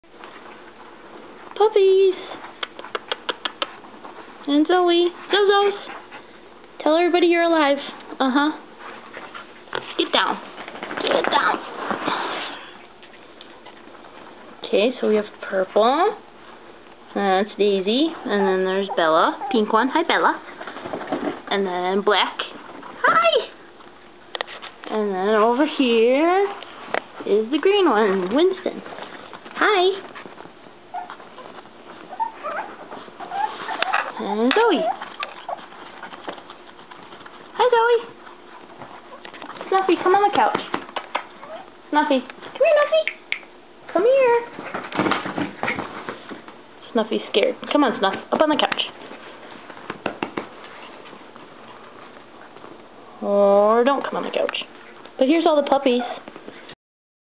Family Photo Attempt
Please excuse the mess! I had just taken the pups for a check up, and somehow managed to get the groceries in the house, Imagine that!